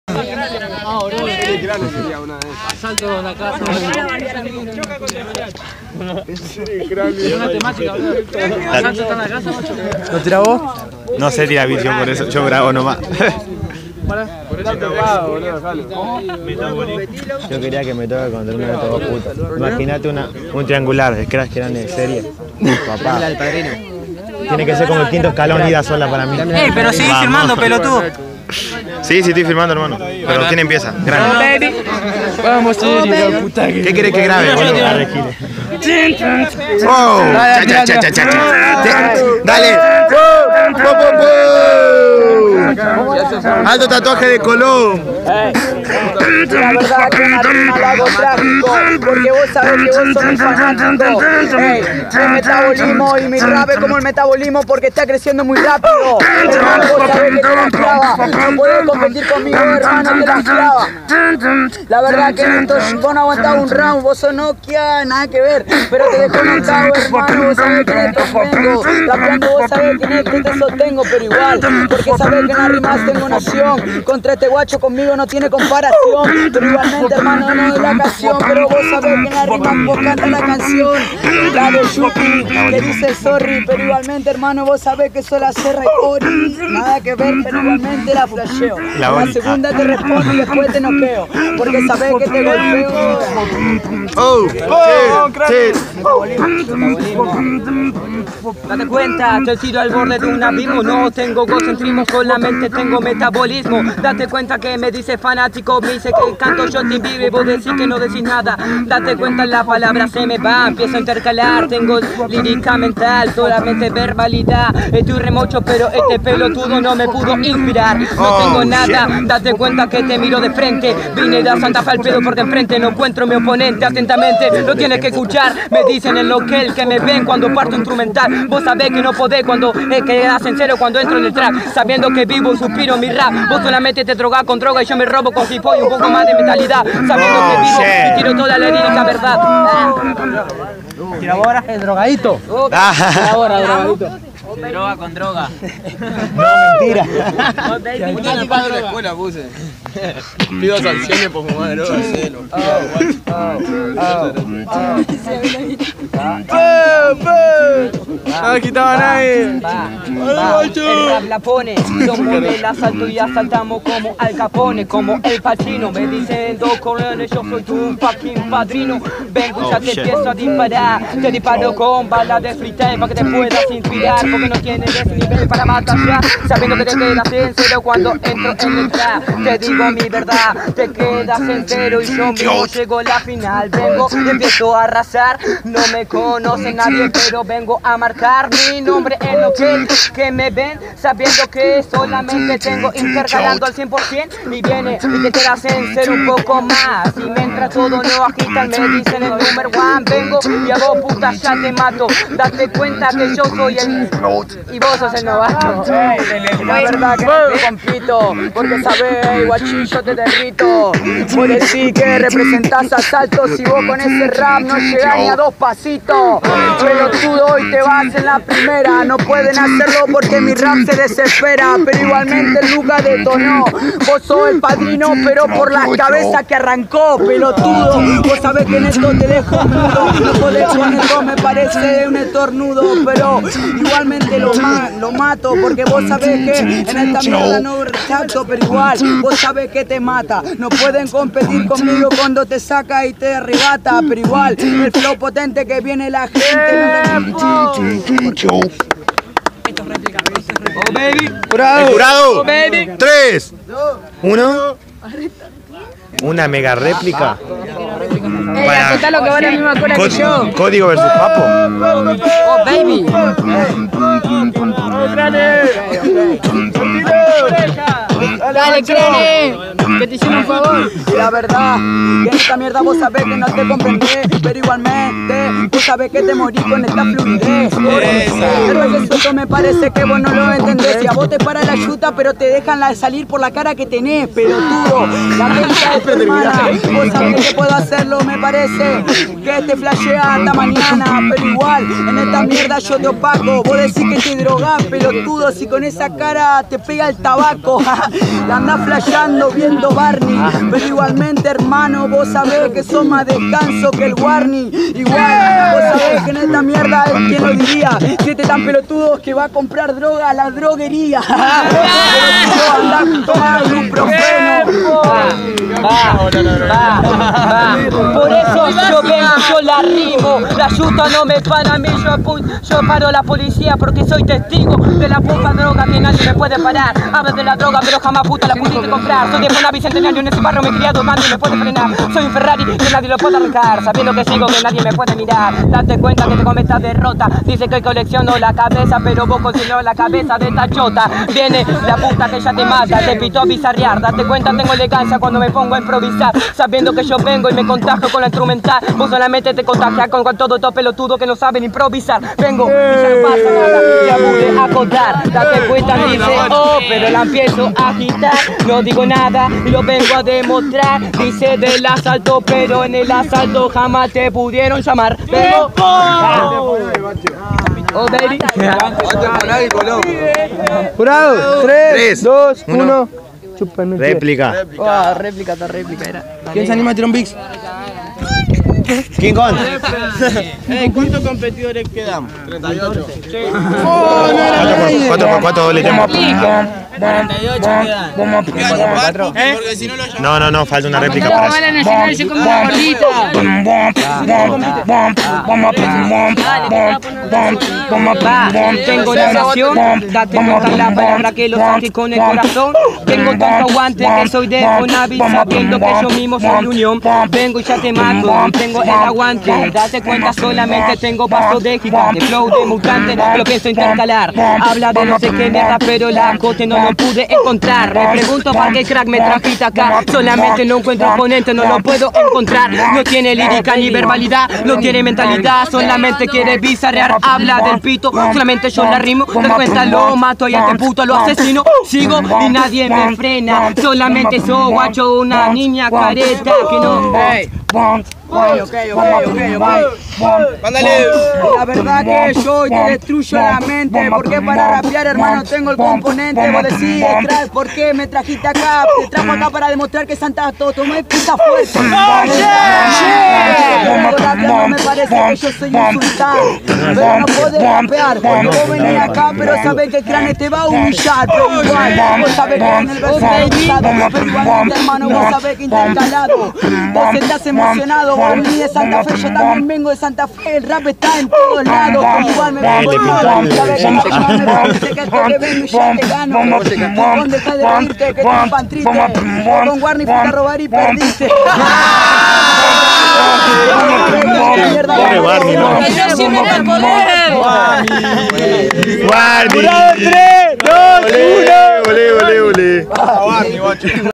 Nokell vs Cranne 8vo Rap Clique 14ta Edicion
Bueno , Salio la décimo cuarta edición de Rap Clique , se hace en la bajada del puente carretero (Santo Tomé)
Domingo 17 a las 19:00
1 vs 1 CON TEMÁTICA , Si hay replica es libre (SIN TEMÁTICA).
Compiten todos..

Jurado: Pupy - Gaby - Lea

Temática:  Metabolismo

Ganador: Cranne